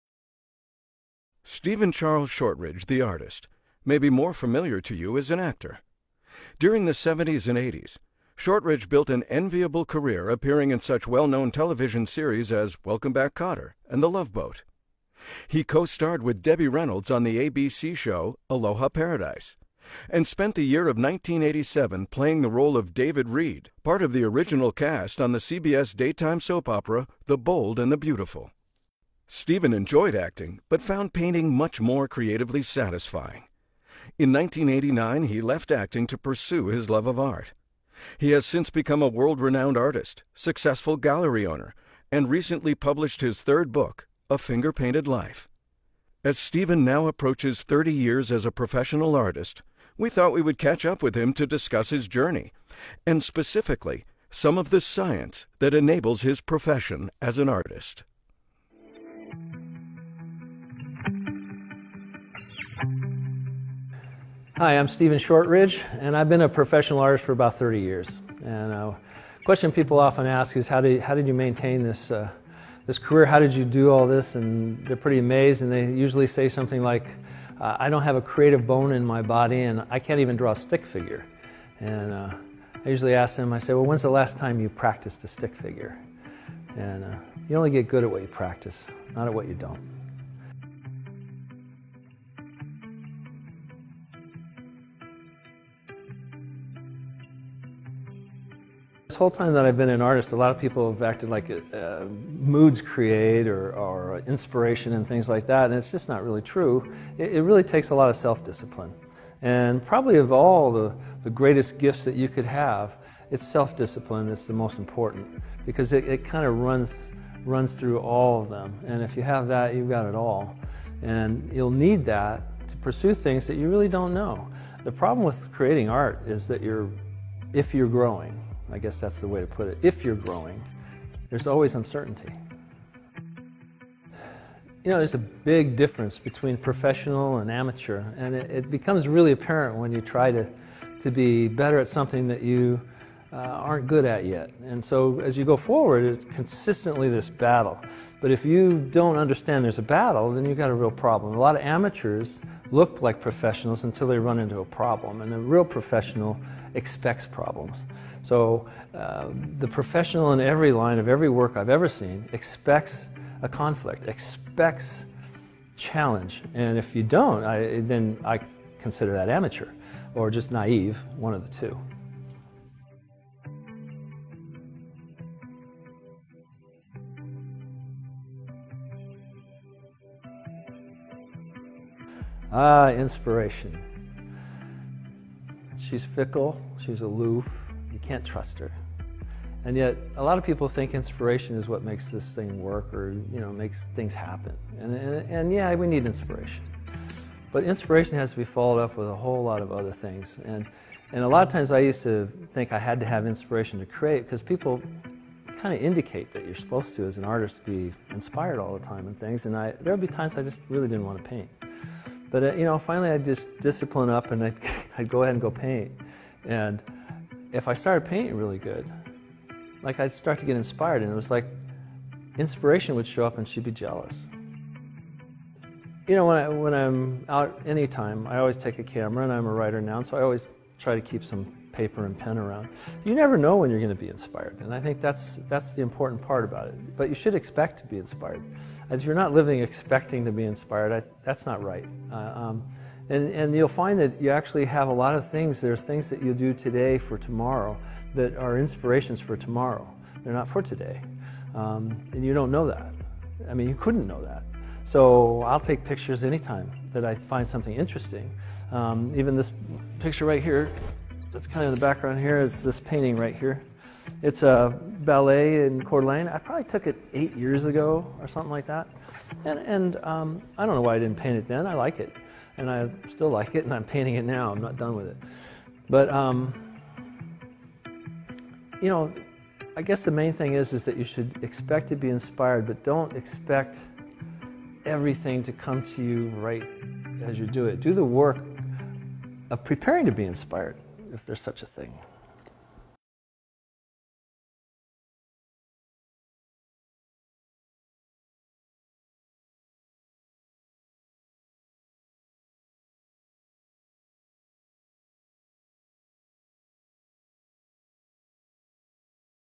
An Interview with Stephen Shortridge
I was fortunate and honored to be interviewed by Motorola to discuss my career and the correlation between art and other professions.
I had a blast doing this video!